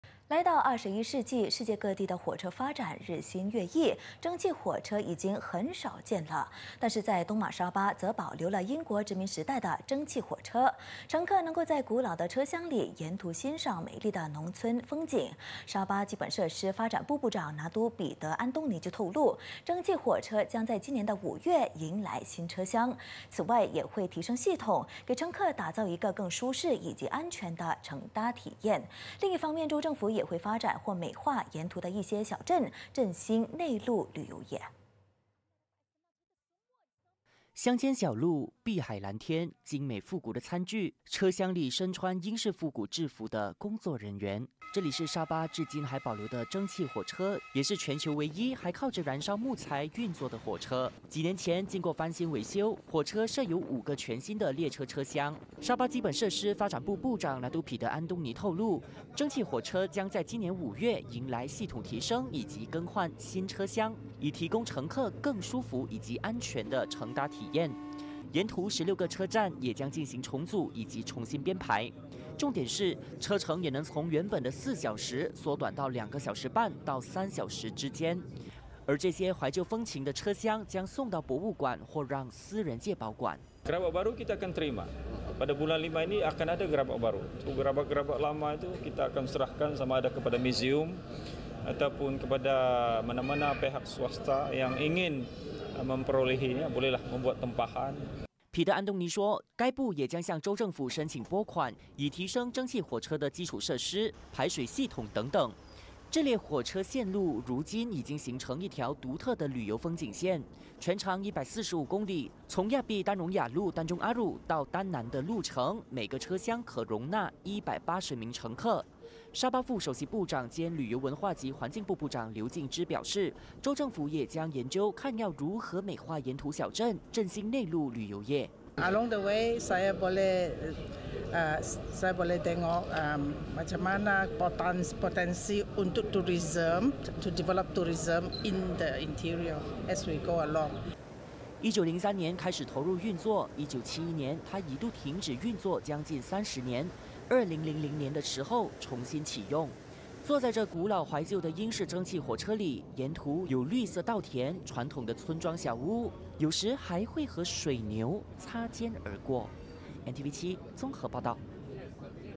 百年英式蒸汽火车 穿越时光开展沙巴复古之旅
来到21世纪，世界各地的火车发展日新月异，“蒸汽火车”已经很少见了！但在东马沙巴，则保留了英国殖民时代的蒸汽火车。乘客能在古老的车厢里沿途欣赏美丽农村风景。沙巴基本设施发展部部长拿督彼得安东尼透露，蒸汽火车将在今年5月引来新车厢；此外也会提升系统， 给乘客打造一个更舒适、以及安全的乘搭经验。另一方面， 州政府也会发展或美化沿途的一些小镇， 振兴内陆旅游业。